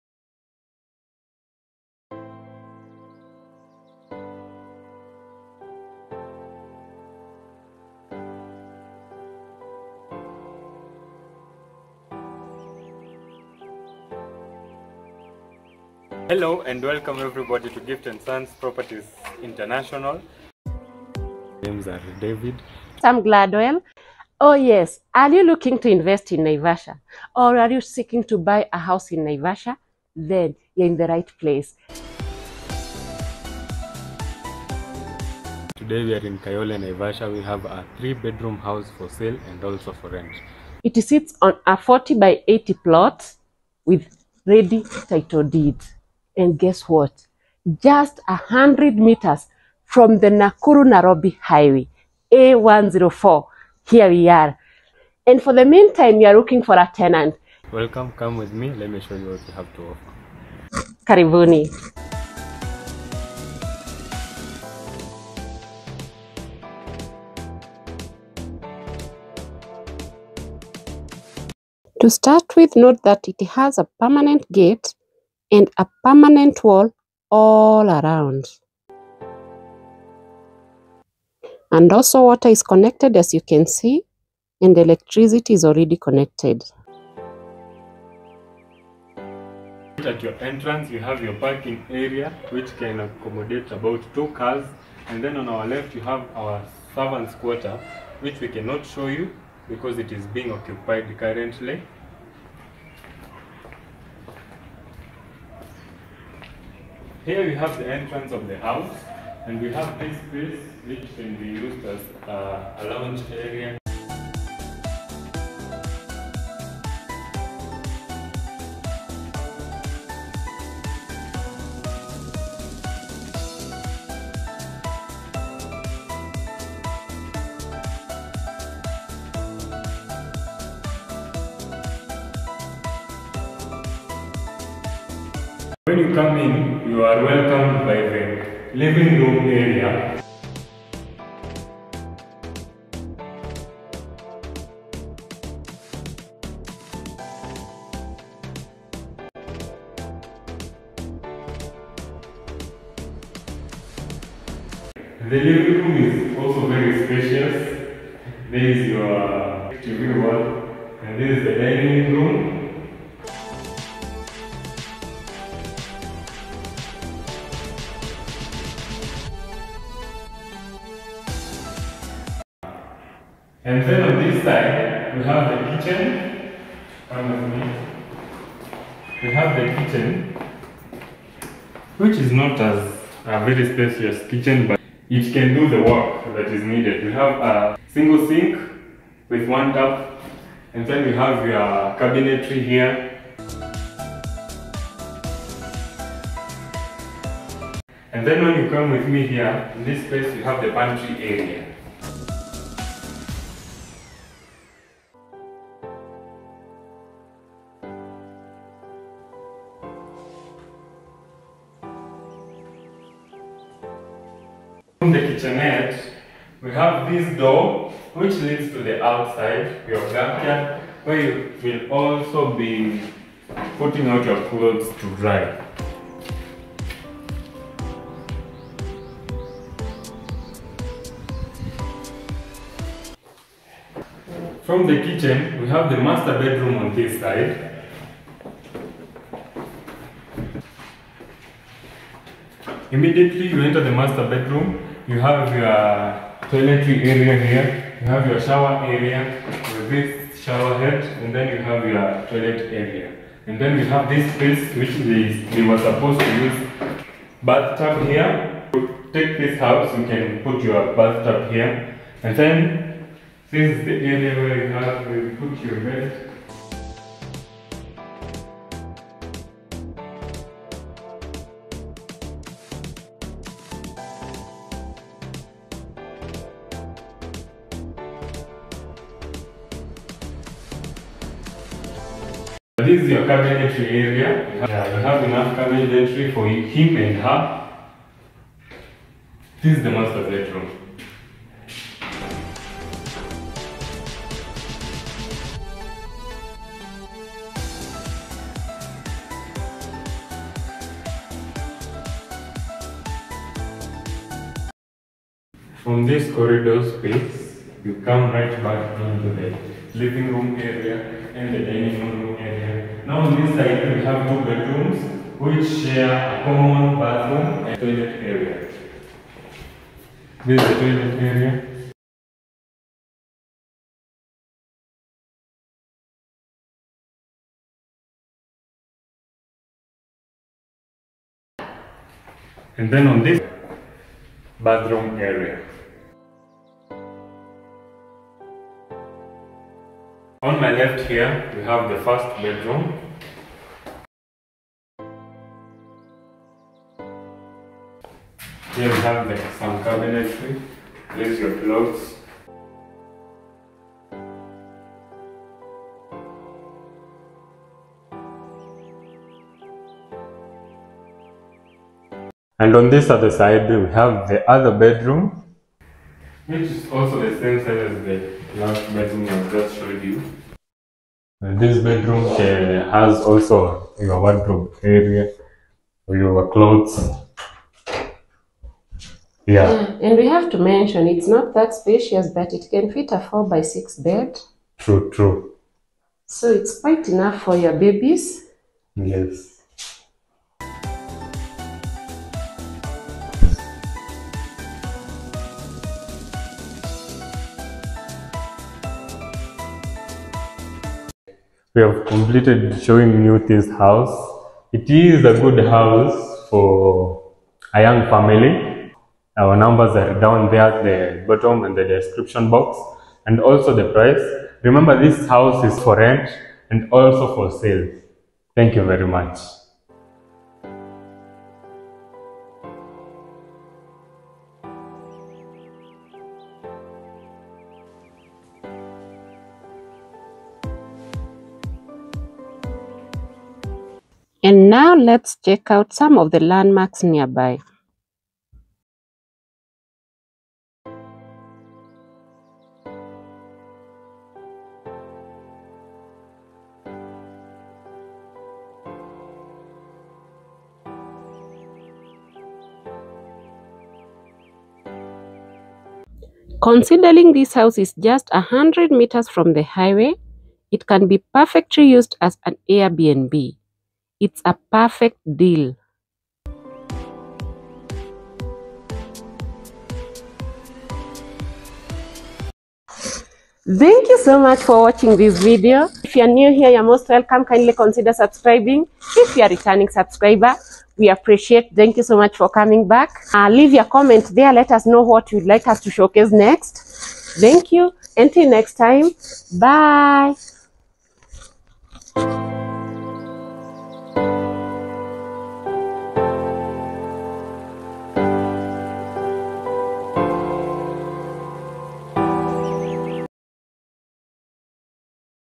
Inside a Kshs 7million 3bedroom house On sale In Naivasha Kenya!!
Asking price Kshs 7million negotiable
With clean title deed
A 3bedroom master ensuite
Sitting on a 40by80 plot size in Kayole Naivasha
Just 100 mtrs from the Nairobi Nakuru highway
Renting out at Kshs 20,000 per month
Can be used as an Air BnB